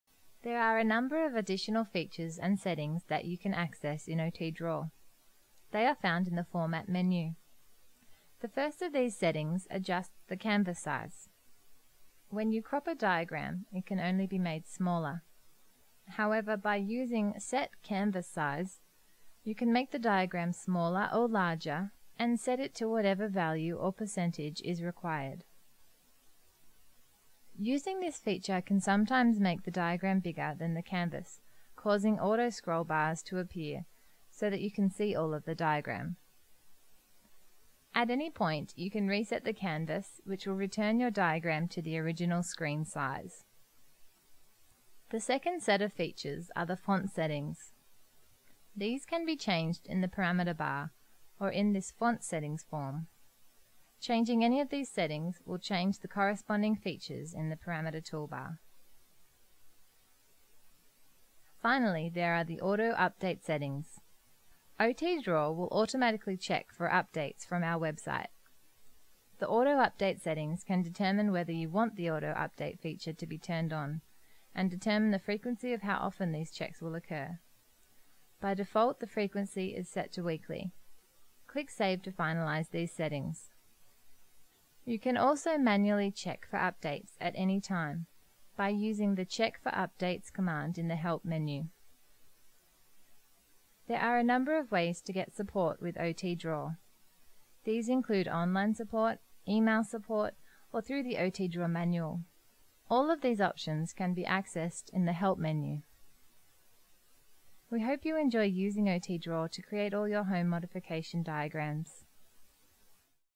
OT Draw Tutorial 12 - Settings and Support
12 of 12 Tutorial Videos for OT Draw.
This video outlines the various features and setting to customise your OT Draw experience.